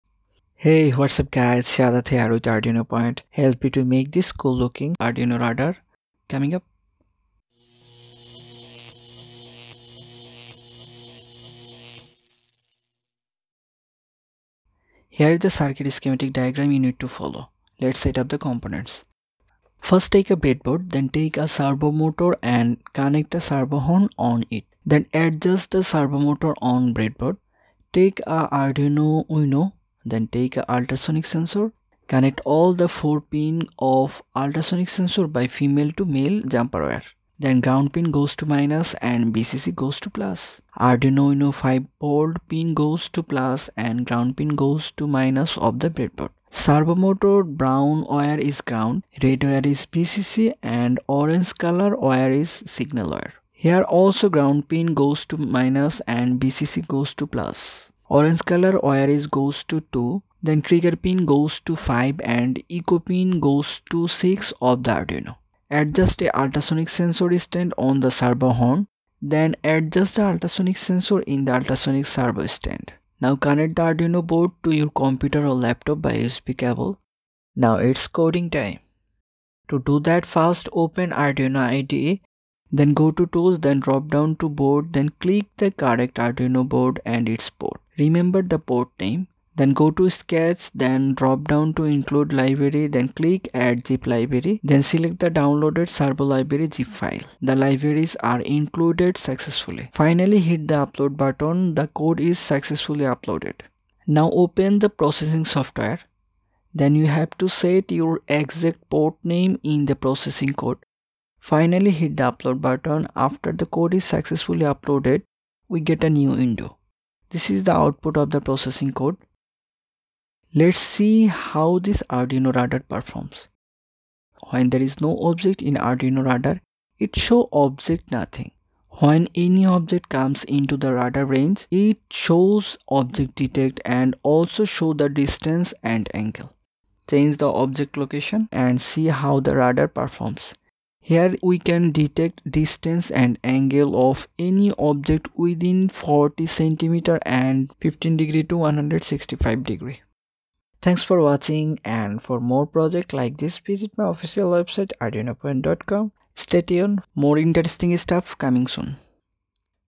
Arduino Radar Project 180 Degree Using HC-SR04 Ultrasonic Sensor
In this project, I will show you how to make a radar using the Arduino and the Processing Development Environment. This project is excellent for showing the distance and angle of any objects within the radar range.
This radar fits any screen resolution.

Components for the Arduino Radar Project
1. Arduino Uno × 1
2. Ultrasonic Sensor HC-SR04 × 1
3. Servo Motor × 1
4. Breadboard × 1
5. Jumper wires kit × 1